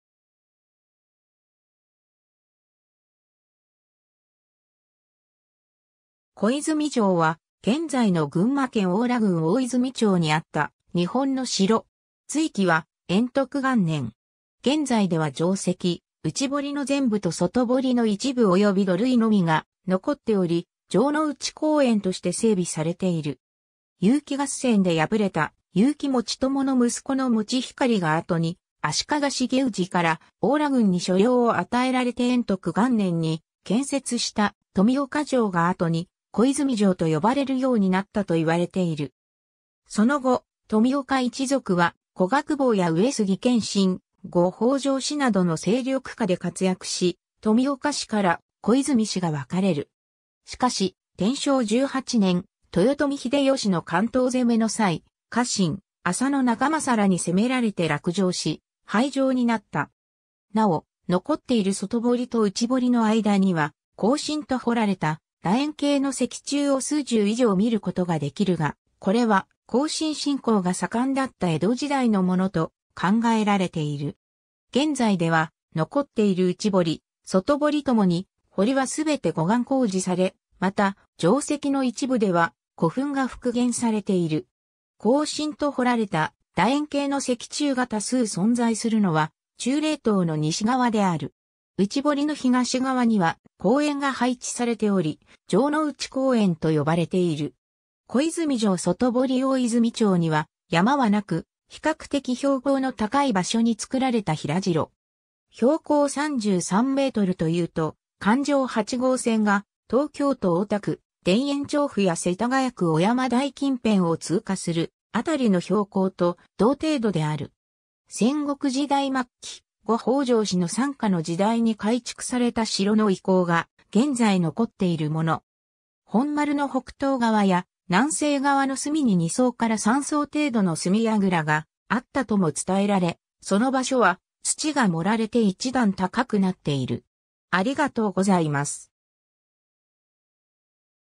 小泉城
小泉城（こいずみじょう）は現在の群馬県邑楽郡大泉町にあった日本の城。
築城は、延徳元年（1489年）。
現在では城跡(内堀の全部と外堀の一部および土塁）のみが残っており、城之内公園（しろのうちこうえん）として整備されている。
 結城合戦で敗れた結城持朝の息子の持光（後の富岡主税介直光）が後に足利成氏から邑楽郡に所領を与えられて延徳元年（1489年）に建設した富岡城が後に小泉城と呼ばれるようになったと言われている。
その後、富岡一族は古河公方や上杉謙信、後北条氏などの勢力下で活躍し、富岡氏から小泉氏が分かれる。
しかし、天正18年（1590年）、豊臣秀吉の関東攻めの際、家臣浅野長政らに攻められて落城し、廃城になった。
なお、残っている外堀と内堀の間には「庚申」と彫られた楕円形の石柱を数十以上見ることができるが、これは庚申信仰が盛んだった江戸時代のものと考えられている。
 現在では、残っている内堀、外堀ともに、堀はすべて護岸工事され、また、城跡の一部では古墳が復元されている。
「庚申」と彫られた楕円形の石柱が多数存在するのは、忠霊塔の西側である。
内堀の東側には公園が配置されており、城之内公園と呼ばれている。
 小泉城外堀（2012年11月） 大泉町には山はなく、比較的標高の高い(33m近辺)場所に作られた平城。
標高33mというと環状八号線が東京都大田区田園調布や世田谷区尾山台近辺を通過するあたりの標高と同程度である。
 戦国時代末期、後北条氏の傘下の時代に改築された城の遺構が現在残っているもの。
 本丸の北東側や南西側の隅に2層から3層程度の隅櫓があったとも伝えられ、その場所は土が盛られて一段高くなっている。